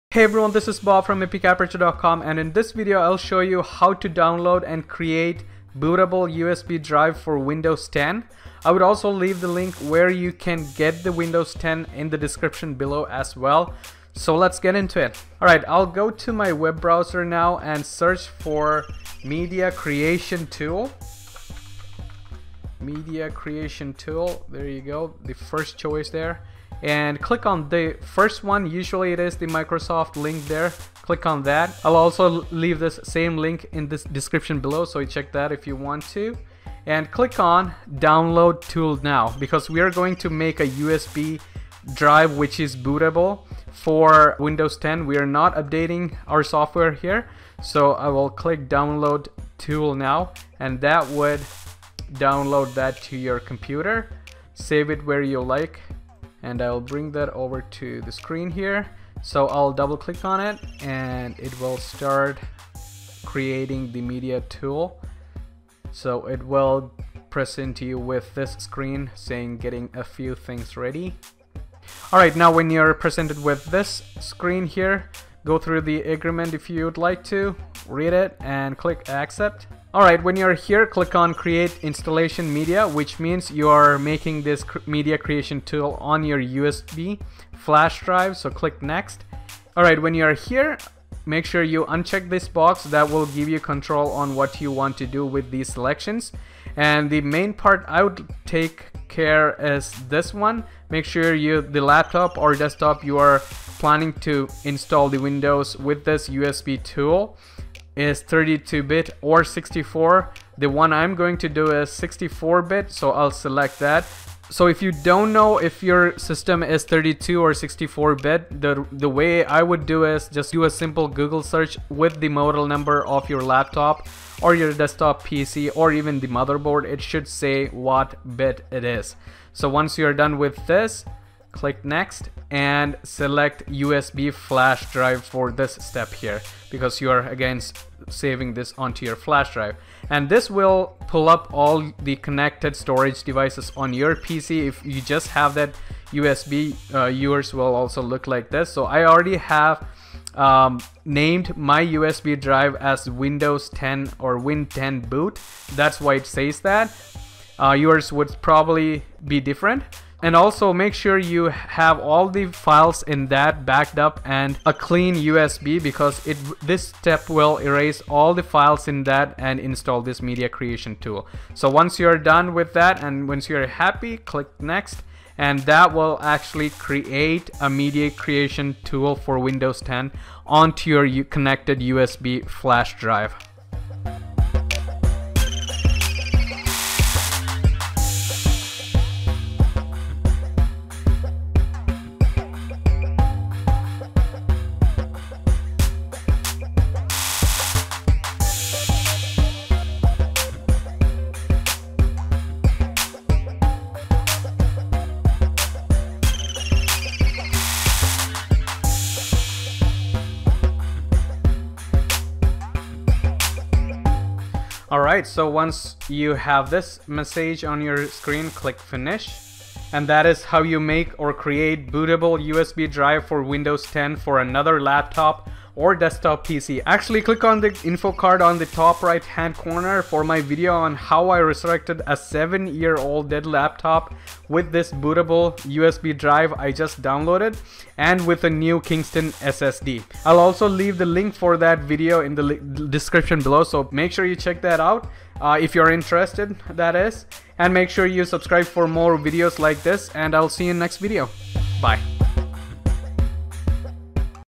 Create Bootable USB Drive For Windows 10
How To Create Bootable USB Drive For Windows 10
Here is the link for the Media Creation Tool for Windows 10
Once you are on the Microsoft Media Creation Tool page for Windows 10, follow the steps I show on this video so that you can successfully create a bootable USB drive for Windows 10 for your system.

I've tried these steps exactly to resurrect one of my old windows laptops to life by moving from HDD to SSD and you may watch that video